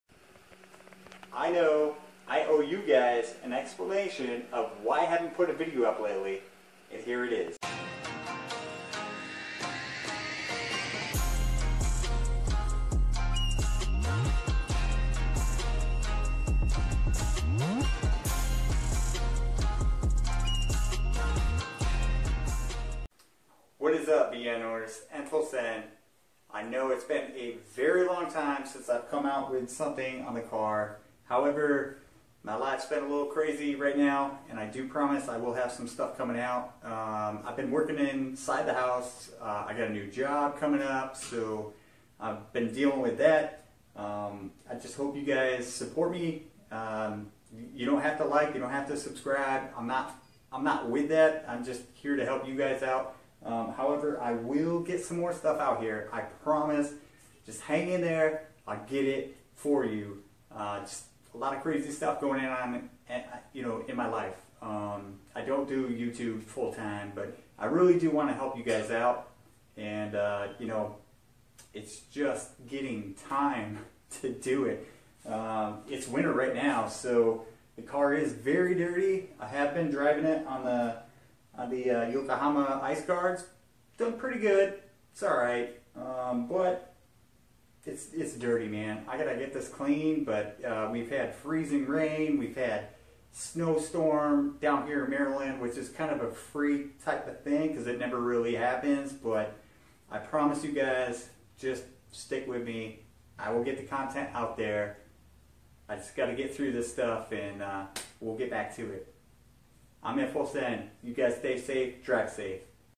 Why has it been awhile?
Explaining why I've not posted in awhile